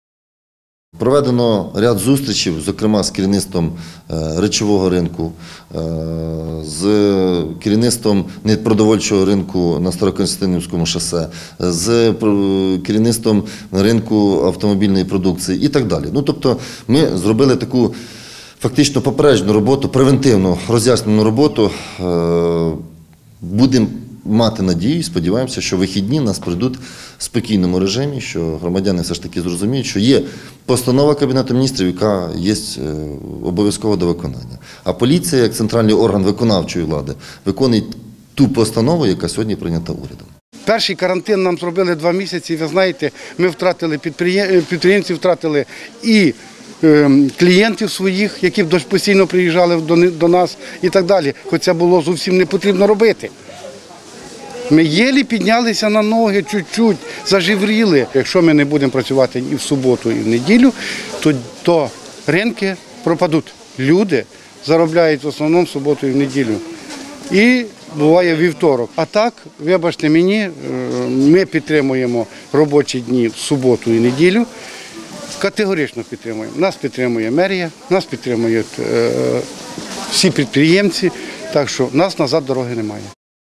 Ринки працюватимуть, попри заборону
Працювати у “карантин вихідного дня” заборонено. Про це підприємцям, які торгують на речових і непродовольчих ринках міста, нагадують поліцейські. І попереджають, що за фактами порушень найближчої суботи та неділі знову складатимуть протоколи. Підприємці, тим часом, не планують ані переносити вихідні, ані зачиняти ятки у суботу та неділю. Кажуть, що лише у ці два дні можуть отримати хоч якийсь прибуток.